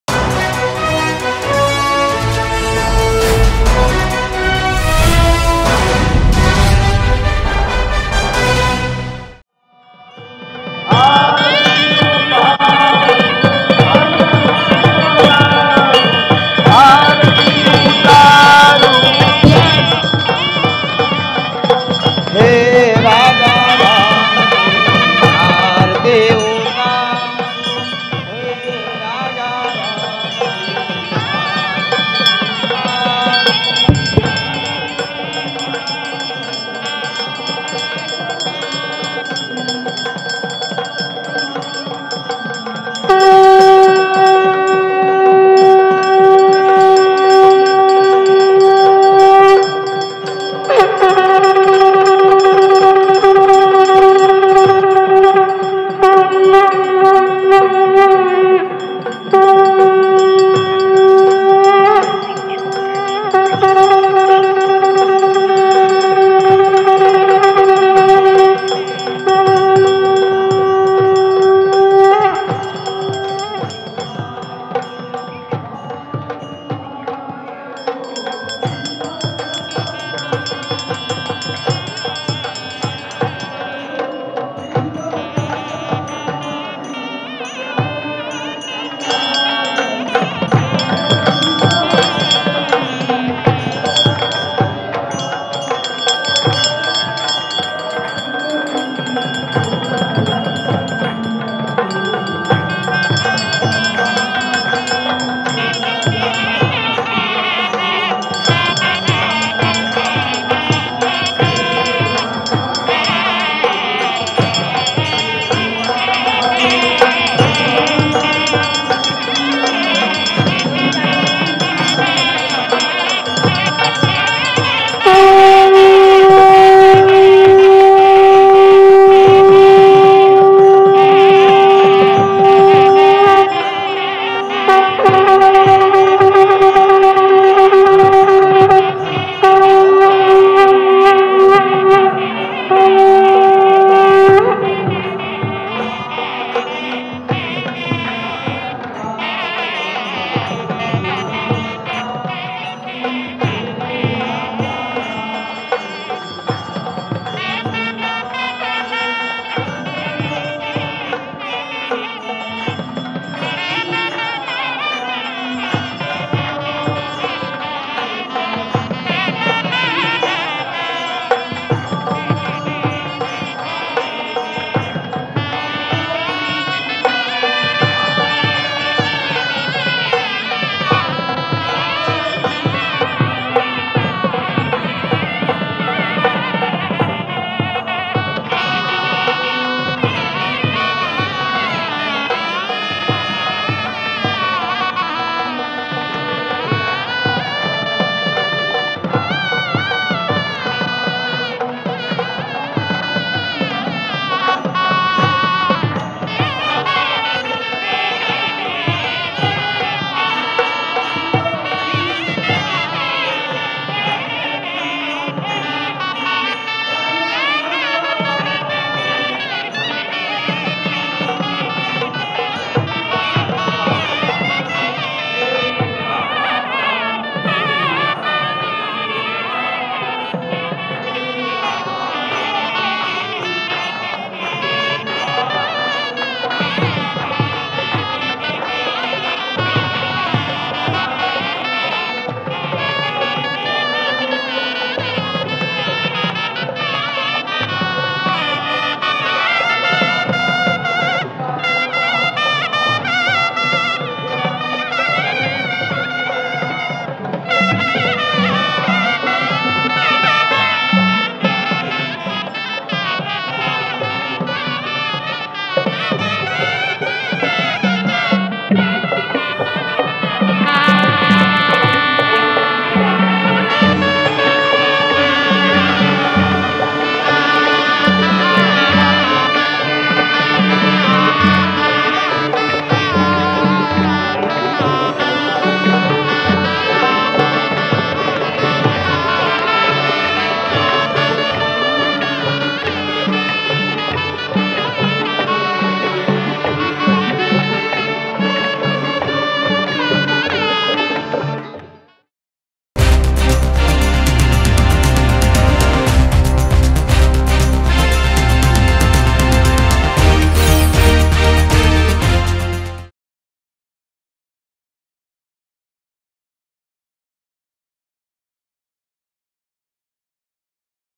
PM Modi Gives Harathi To Shri Ram Lalla Idol | Ayodhya Ram Mandir Pran Pratishtha | Mango News
PM Modi Gives Harathi To Shri Ram Lalla Idol.